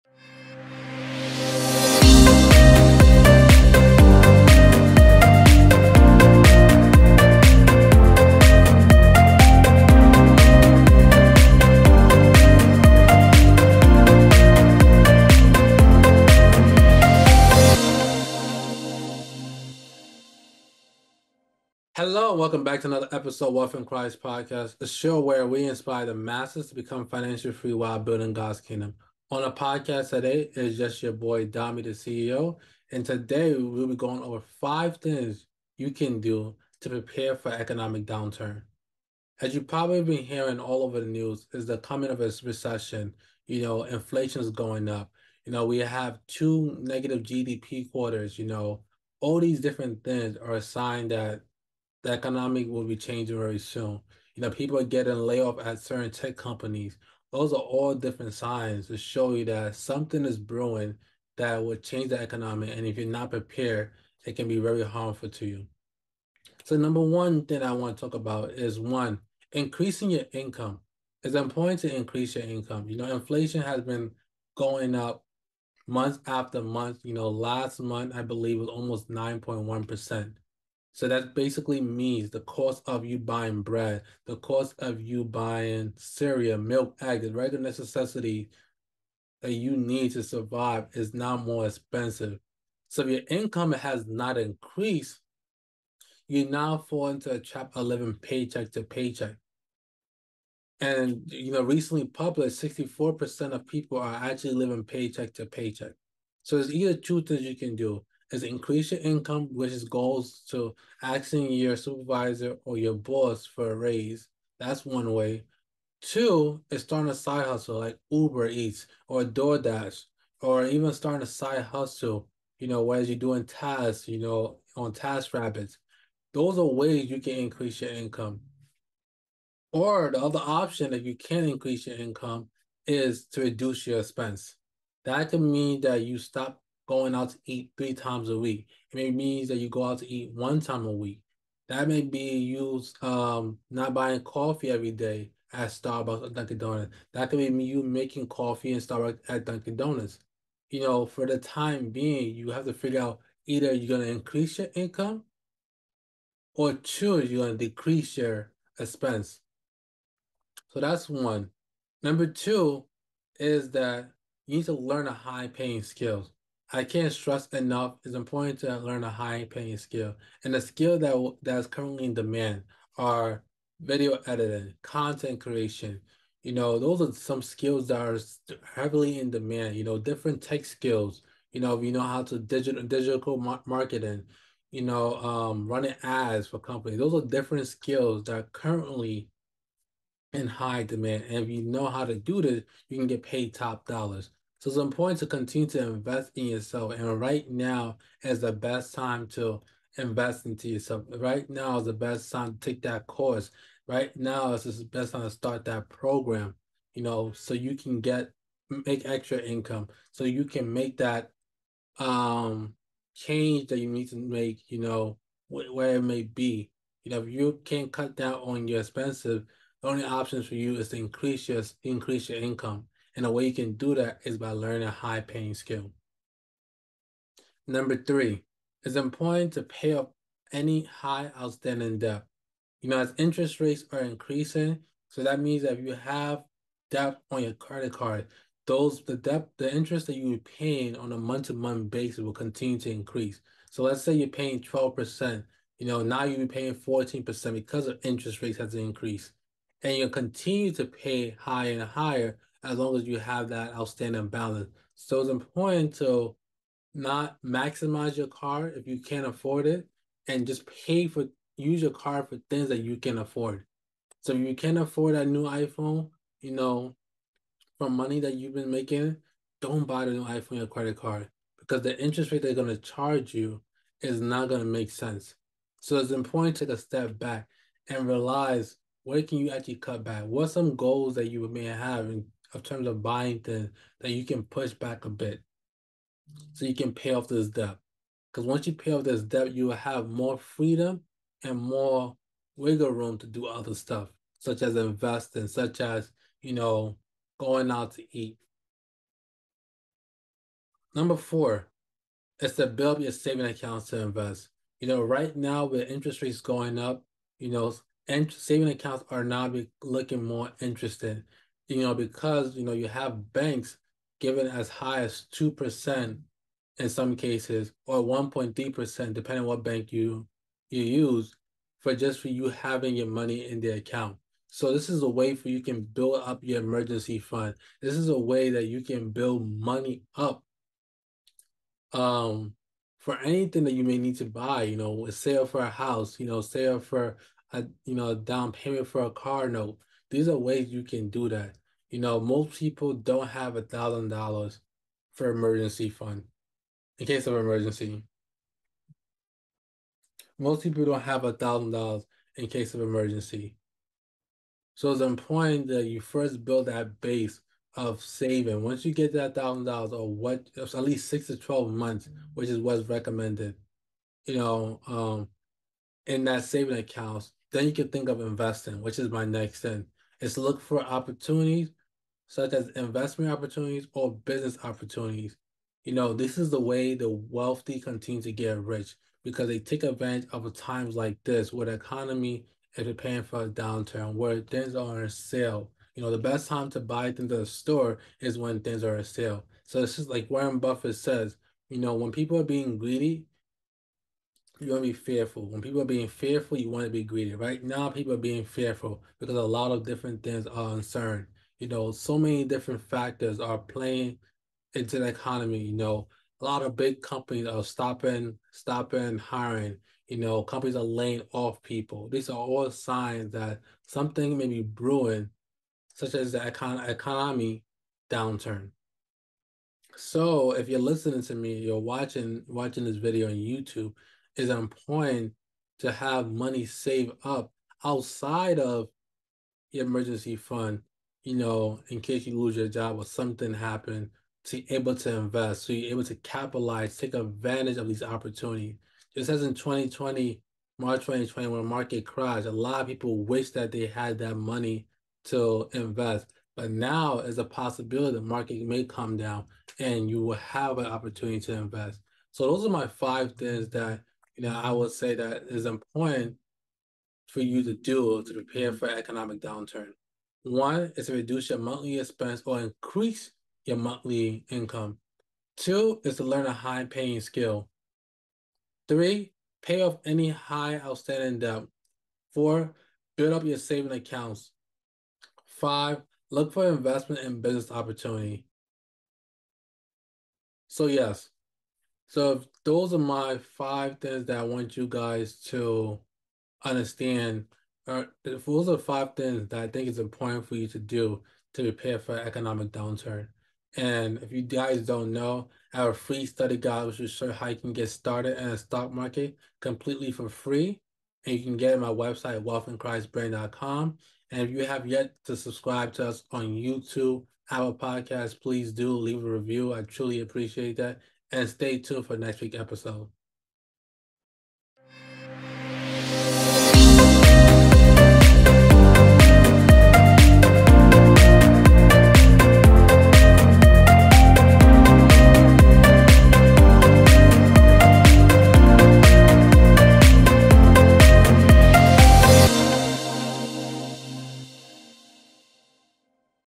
How to prepare for an economy downturn
In episode 75 of the Wealth in Christ podcast, Dami explains how to prepare for an economic downturn in 5 simple steps.